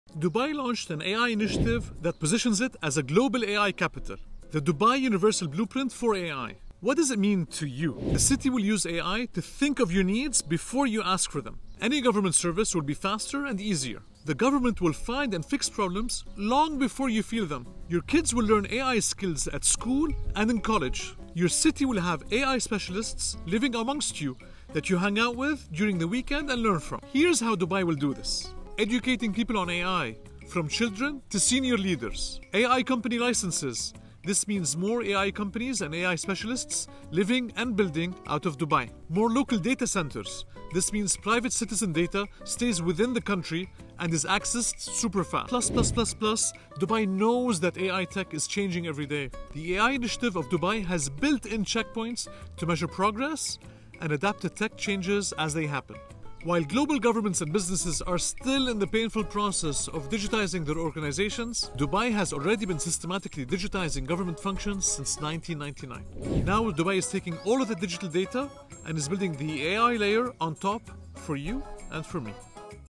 The Dubai Universal Blueprint for AI.
Dubai launched an AI initiative that positions it as a global AI capital. The Dubai Universal Blueprint for AI.

What does it mean to you?

- The city will use AI to think of your needs before you ask for them.
- ⁠Any government service will be faster and easier.
- ⁠The government will find and fix problems long before you feel them.

While global governments and businesses are still in the painful process of digitizing their organizations, Dubai has already been systematically digitizing government functions since 1999.

Now Dubai is taking all of the digital data and is building the AI layer on top for you and for me.